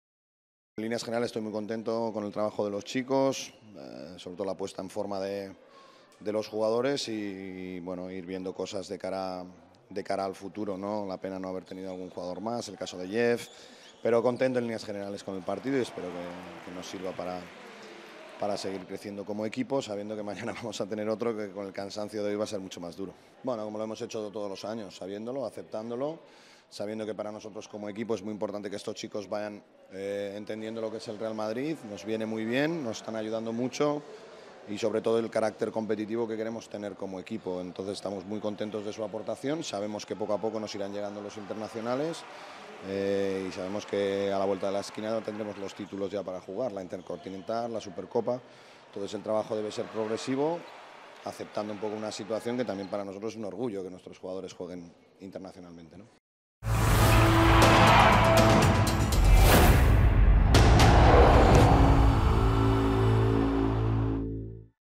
Laso: “Estoy contento porque el primer día siempre es muy difícil”
“El equipo ha jugado bien porque todos hemos luchado”, afirmó Carroll.